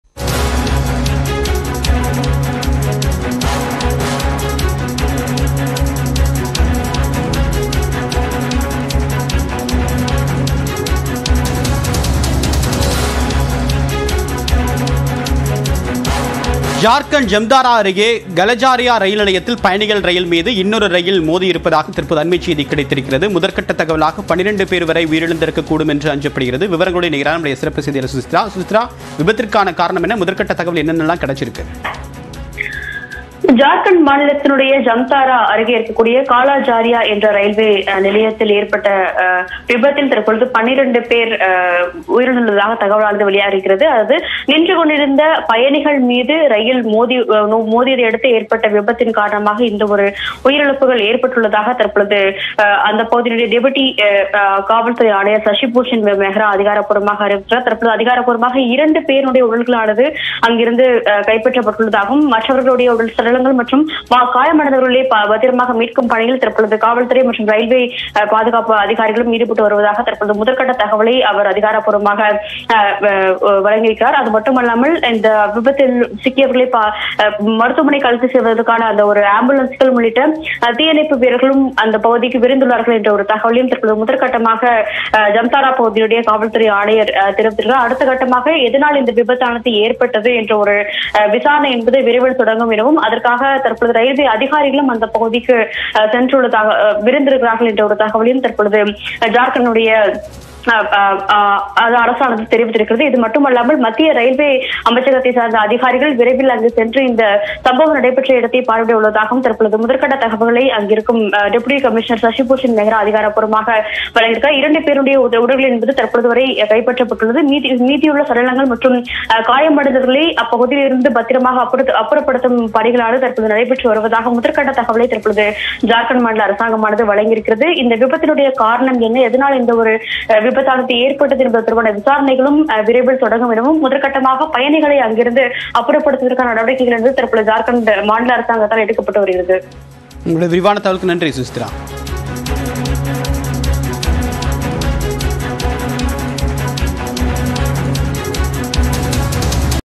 Breaking News | ஜார்க்கண்ட்டில் பயணிகள் மீது மோதிய ரயில் - 12 பேர் உயிரிழந்திருக்கலாம் என தகவல்
ஜார்க்கண்ட்: ஜம்தாரா அருகே கலஜாரியா ரயில் நிலையத்தில் பயணிகள் மீது மோதிய ரயில்.  செய்திகளை துல்லியமாகவும் துரிதமாகவும் வழங்கும் தமிழகத்தின் முதன்மை செய்தி தொலைக்காட்சி உங்கள் நியூஸ்18 தமிழ்நாடு.